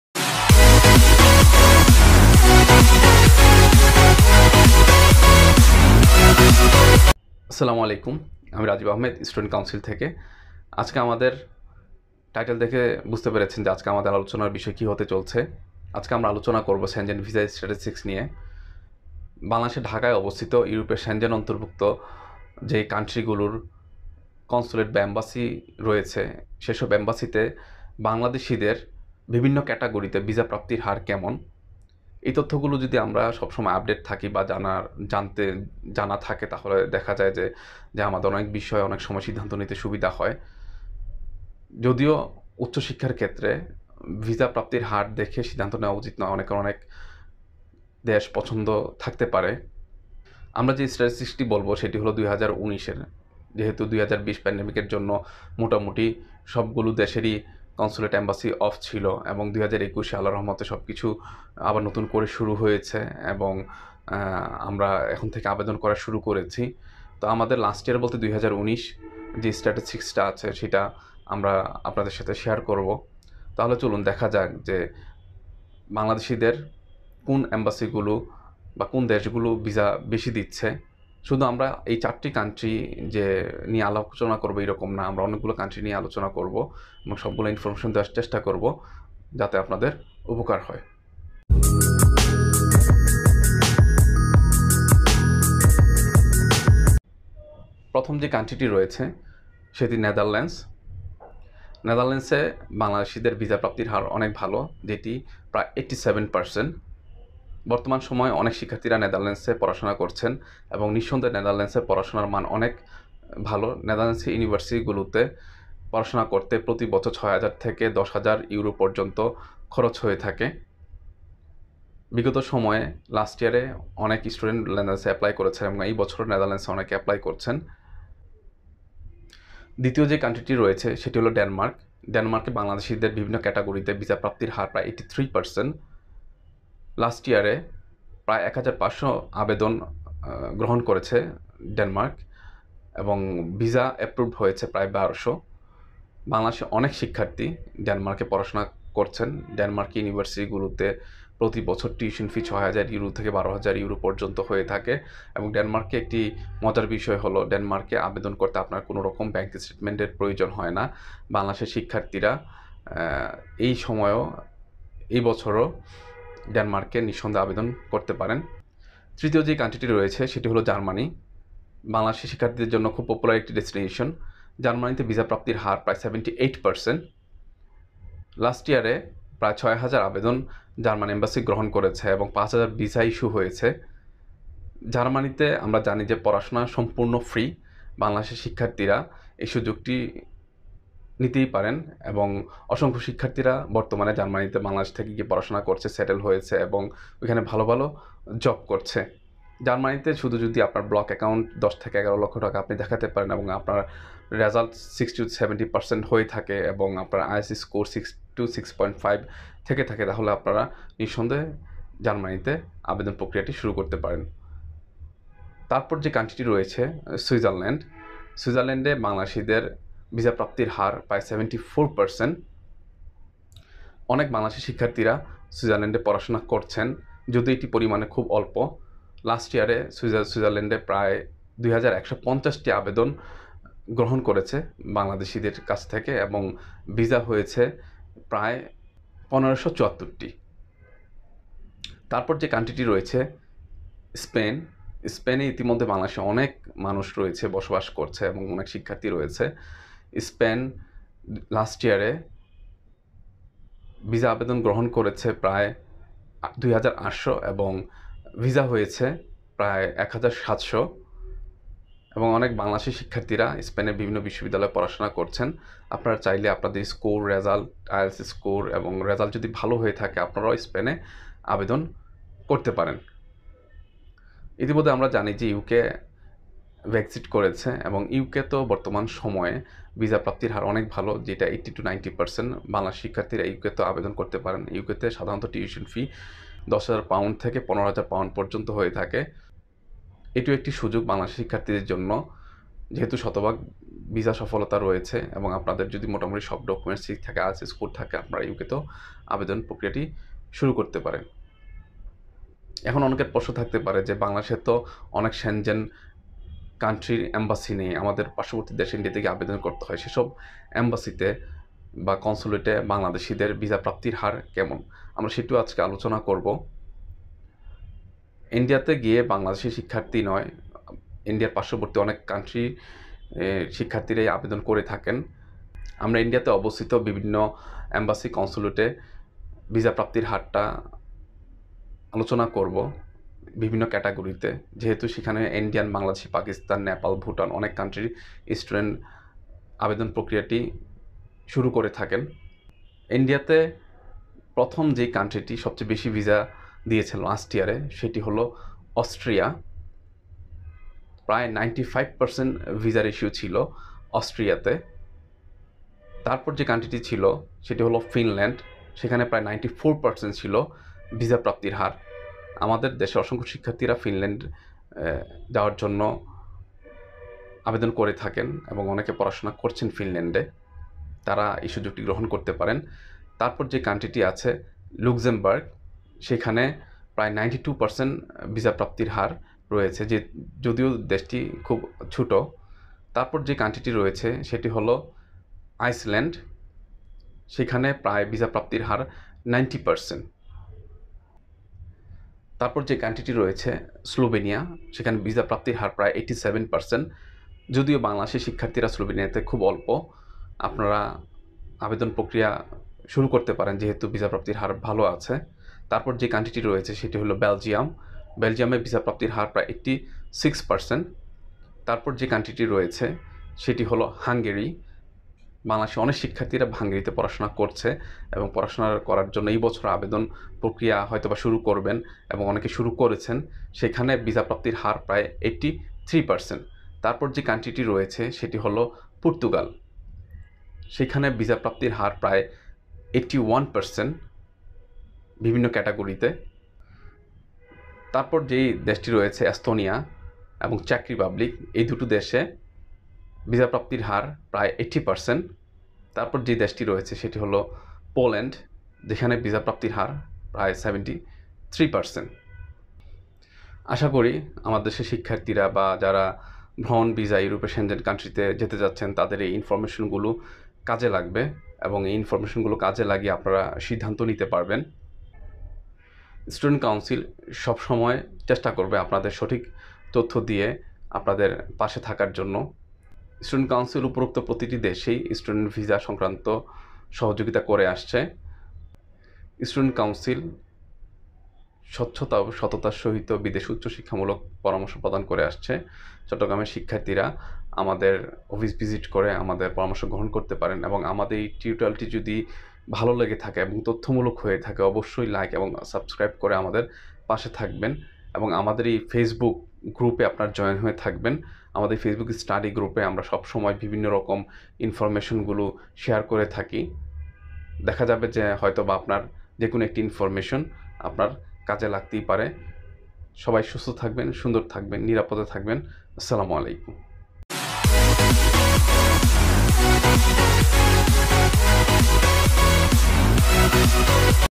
ইউরোপের যেই দেশগুলোতে স্টুডেন্ট ভিসা বেশি হয় | EUROPE-2021|STUDENTS VISA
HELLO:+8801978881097

NETHERLANDS-87%
DENMARK-83%
GERMANY-78%
SWITZERLAND-74%
SPAIN-69%

AUSTRIA-95%
FINLAND-94%
LUXEMBOURG-92%
ICELAND-90%
SLOVENIA-87%
BELGIUM-86%
HUNGARY-83%
PORTUGAL-81%
ESTONIA-80%
CZECH REPUBLIC-80%
POLAND-73%